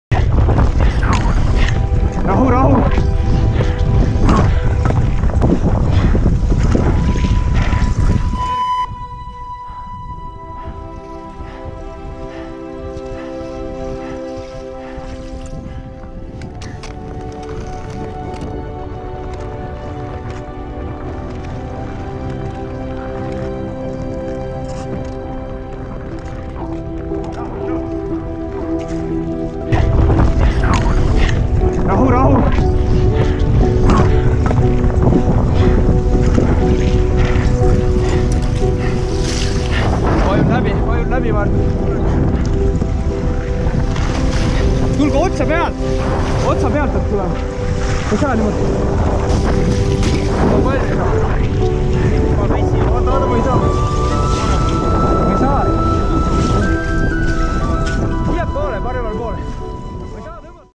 Un pêcheur estonien reste coincé dans un lac gelé
Les secours estoniens ont diffusé les images d'un sauvetage pas comme les autres. Ils ont secouru un pêcheur piégé dans l'eau glacée d'un lac donc la glace s'est effondrée sous ses pieds.